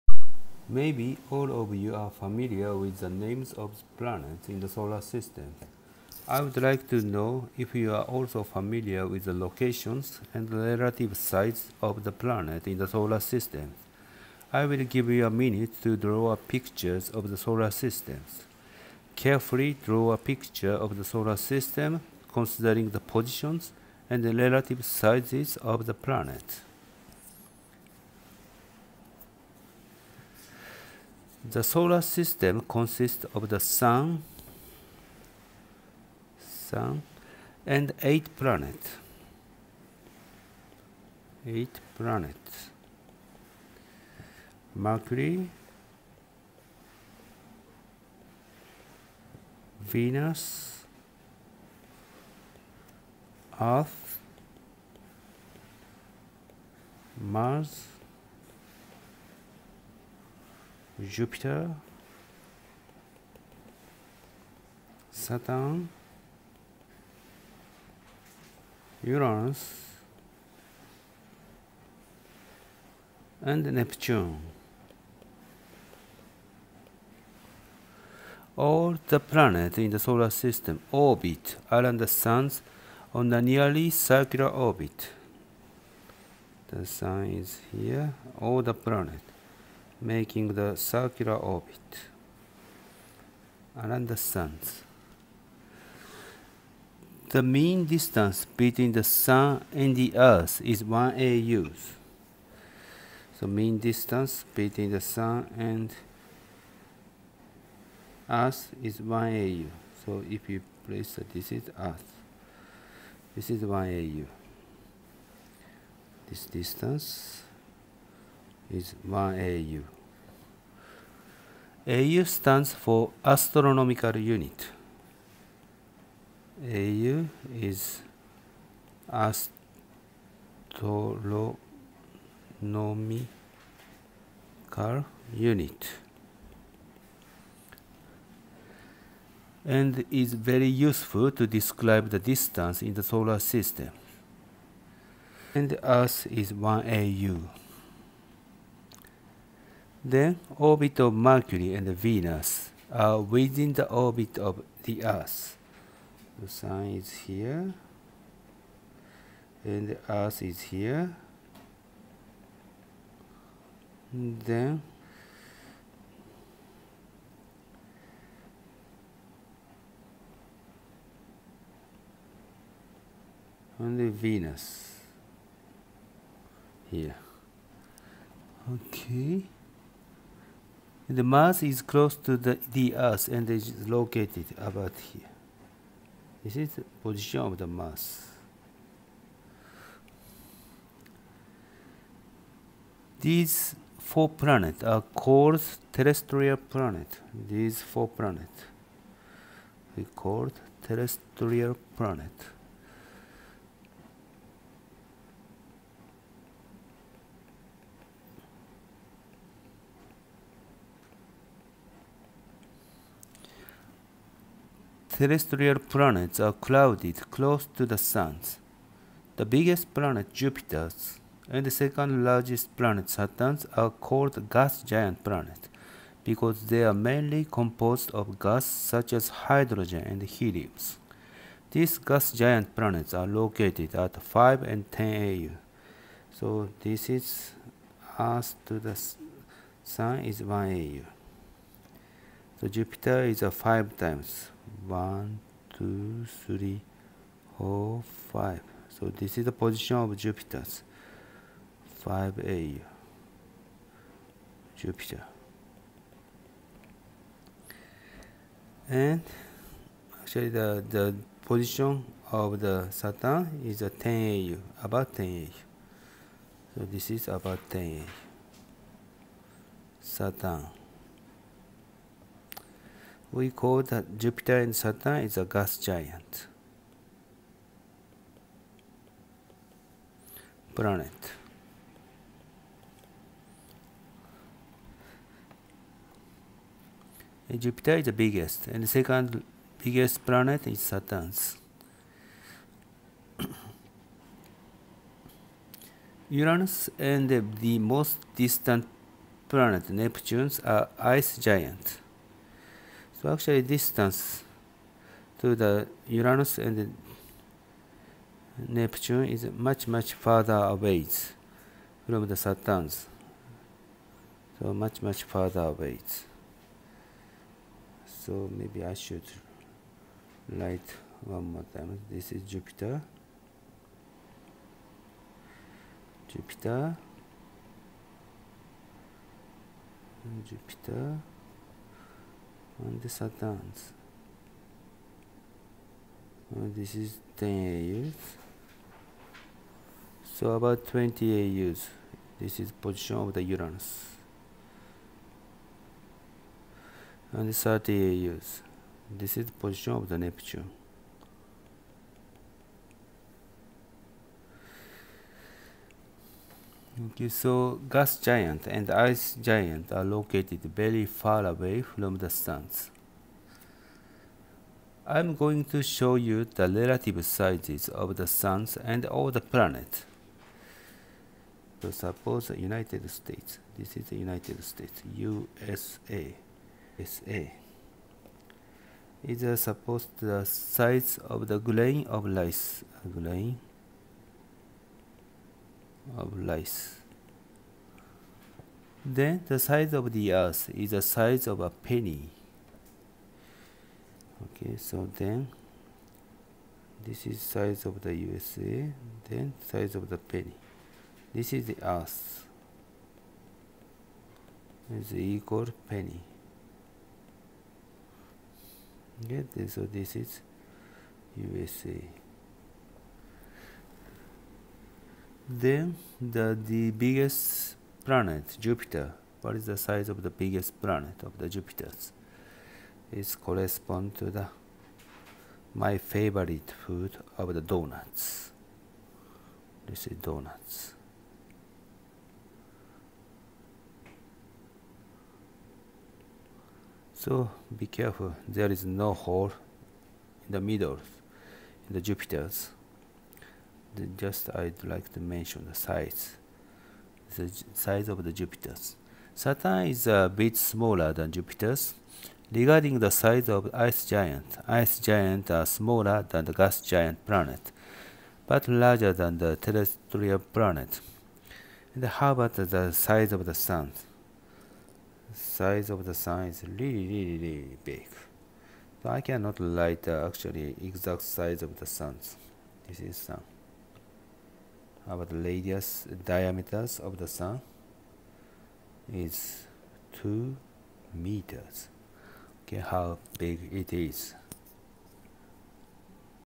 Introduction to Astronomy (Part I, 1.1)
We might think we have already studied our solar system and know a detail of them. As a simple test think of relative sizes of the sun and planets in the solar system and draw them. Also try to draw the positions of the planets when the sun is located at the center.